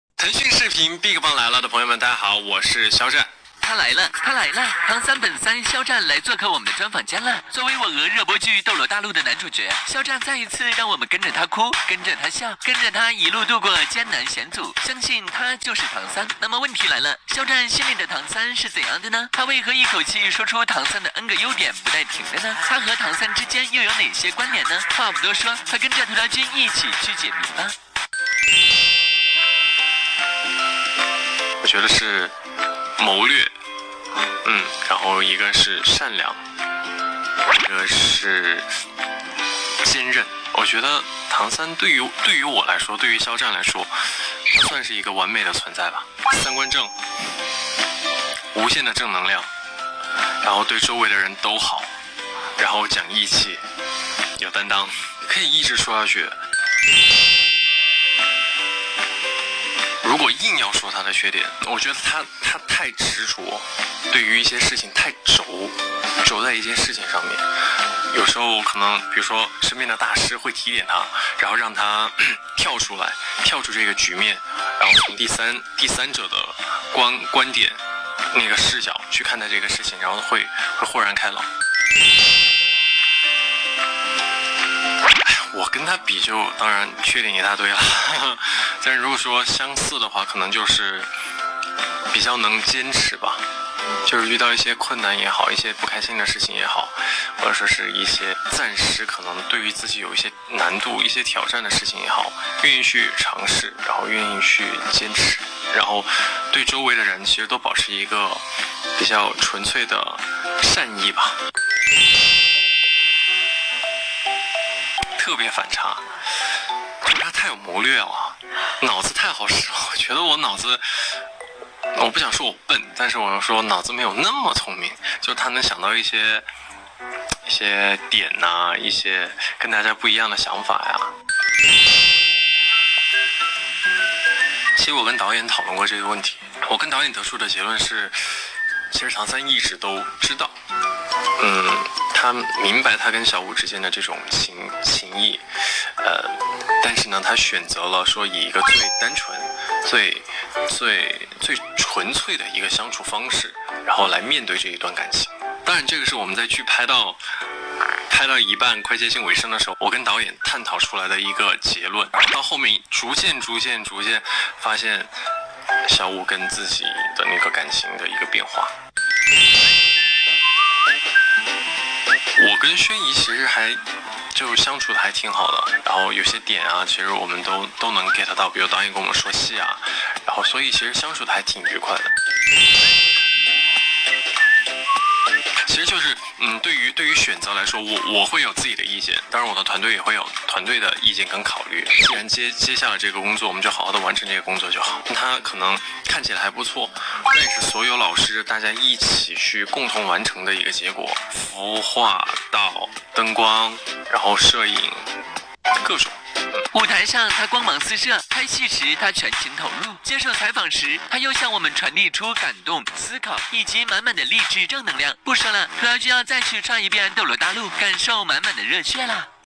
【ENG SUB】Xiao Zhan | Tencent Video Big Bang Lai Le Interview: Perfect Tang San
Xiao Zhan | Tencent Video Big Bang Lai Le Interview: Perfect Tang San